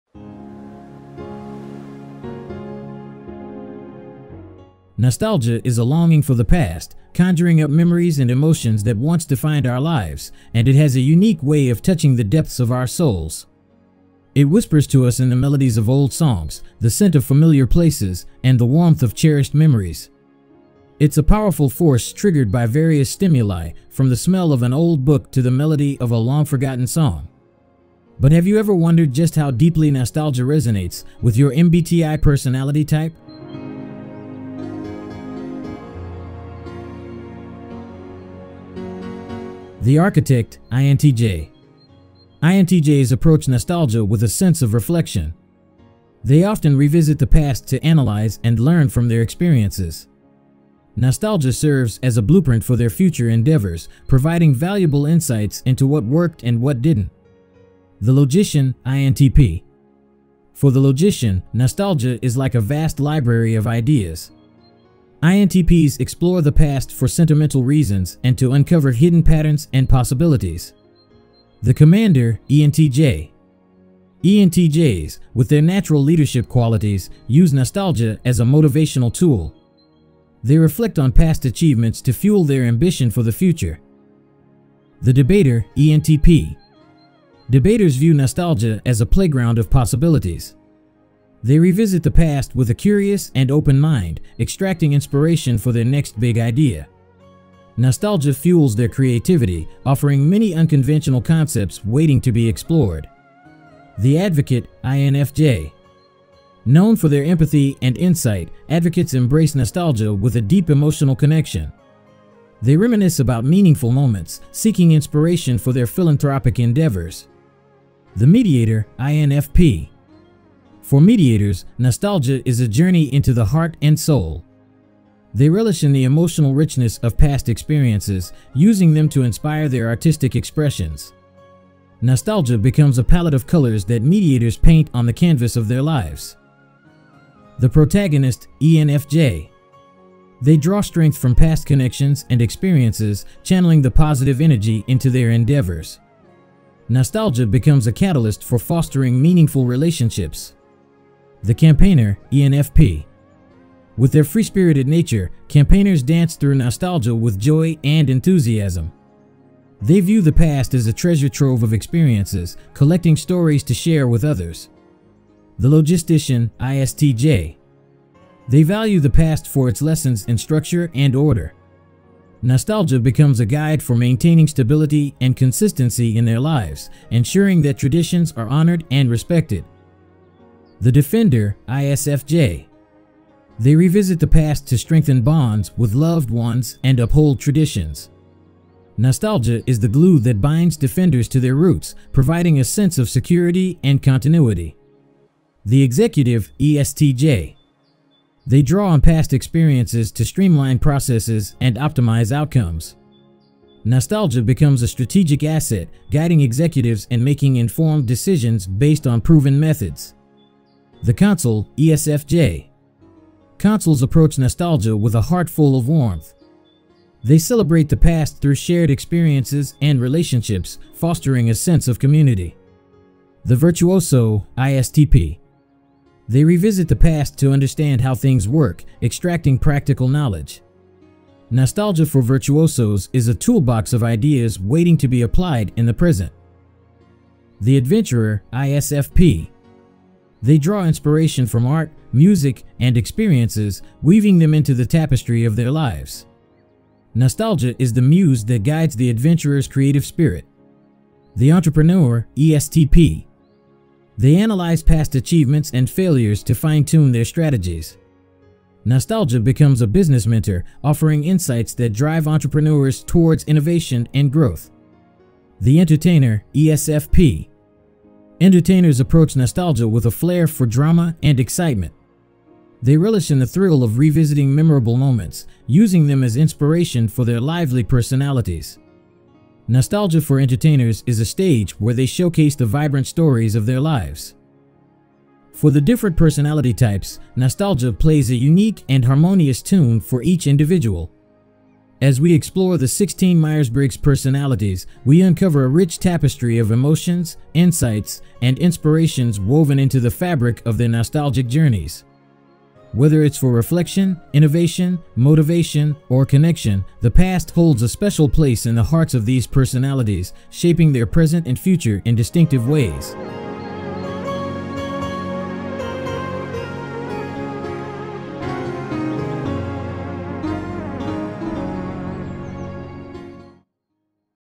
Here Is How Nostalgic You Are According To Your Personality Type
Nostalgia is a longing for the past, conjuring up memories and emotions that once defined our lives, and it has a unique way of touching the depths of our souls. It whispers to us in the melodies of old songs, the scent of familiar places, and the warmth of cherished memories. It's a powerful force triggered by various stimuli, from the smell of an old book to the melody of a long-forgotten song.
But have you ever wondered just how deeply nostalgia resonates, with your MBTI personality type?

THE MAKING OF THIS VIDEO
► Soundtrack:       Psych DIGITAL Studio
► Scripting:        Moses N. Chukwu
► Illustration & Animation:     Moses N. Chukwu